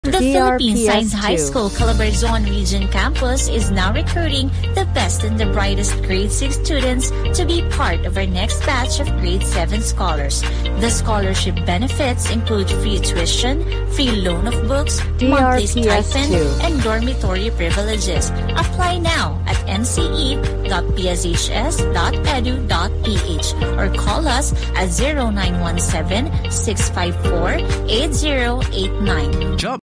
PSHS Calabarzon Radio Ad Q4 2024 30s (Batangas)
Ito po ang Q4 2024 Provincial Radio Ad ng PSHS Calabarzon sa Batangas.

DRPS Two 8796: PSHS Calabarzon Radio Ad Q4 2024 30s (Batangas)